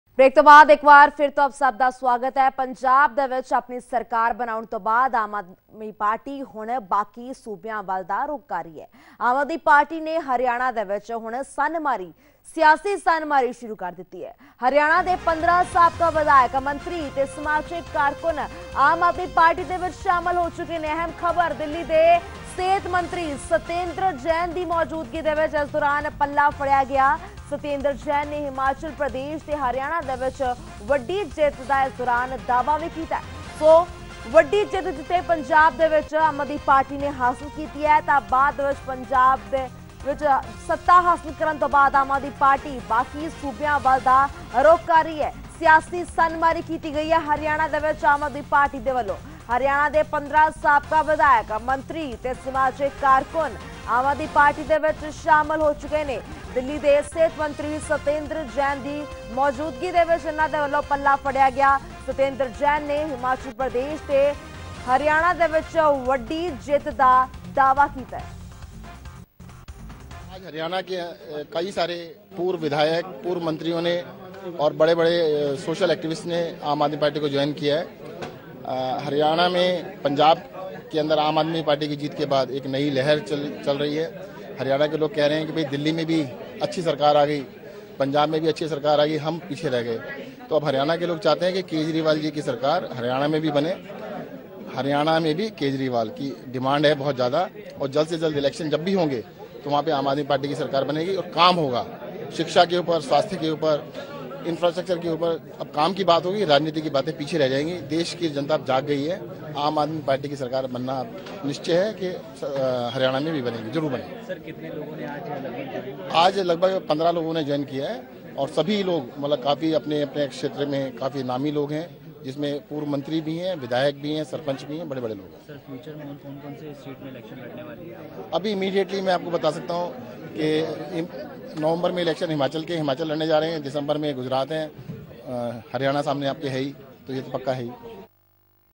'ਆਪ' ਨੇ ਹਰਿਆਣਾ 'ਚ ਸ਼ੁਰੂ ਕੀਤੀ ਸਿਆਸੀ ਸੰਨ੍ਹਮਾਰੀ
ਹਰਿਆਣਾ ਦੇ ਸਾਬਕਾ ਵਿਧਾਇਕ, ਮੰਤਰੀ ਤੇ ਸਮਾਜਿਕ ਕਾਰਕੁੰਨ 'ਆਪ' 'ਚ ਸ਼ਾਮਲ
ਦਿੱਲੀ ਦੇ ਸਿਹਤ ਮੰਤਰੀ ਸਤੇਂਦਰ ਜੇਠ ਦੀ ਮੌਜੂਦਗੀ 'ਚ ਫੜਿਆ 'ਆਪ' ਦਾ ਪੱਲਾ
ਸਤੇਂਦਰ ਜੇਠ ਨੇ ਹਿਮਾਚਲ ਪ੍ਰਦੇਸ਼ ਤੇ ਹਰਿਆਣਾ 'ਚ ਵੱਡੀ ਜਿੱਤ ਦਾ ਕੀਤਾ ਦਾਅਵਾ